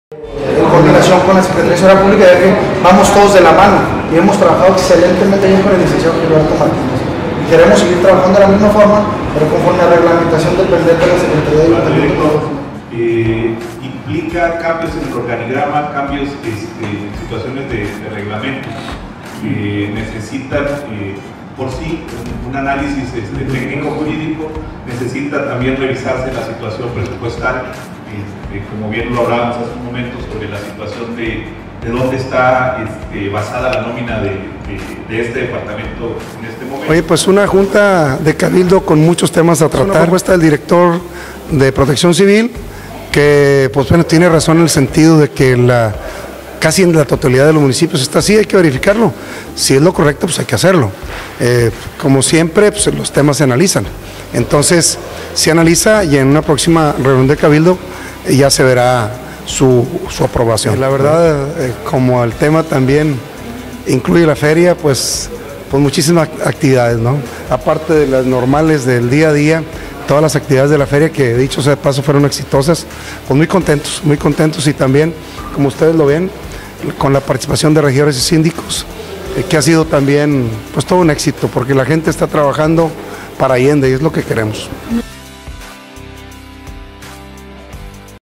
PROPONEN QUE LA DIRECCION DE PROTECCION CIVIL DEPENDA DE LA SECRETARIA DEL AYUNTAMIENTO
Allende.- Con la finalidad de depender de la Secretaria del Ayuntamiento del municipio, el Regidor Pedro Díaz dio lectura a la petición del Director de Protección Civil Rubén Tamez en donde se determina investigar la propuesta y de ser factible su posterior análisis y aprobación.